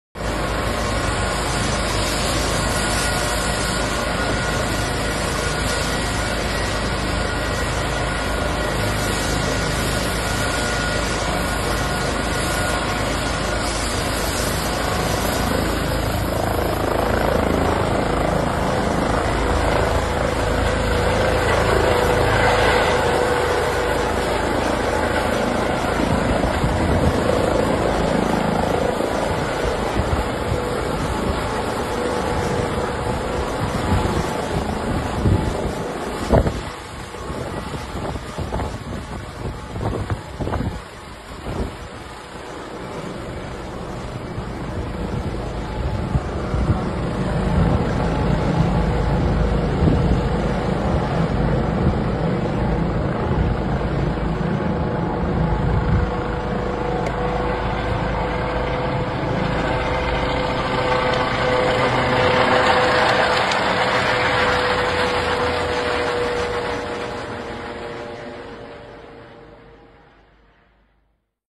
Helikopter LPR w Zawierciu
Helikopter LPR w akcji w Zawierciu na ulicy Nowowierzbowej gdzie miało miejsce pożar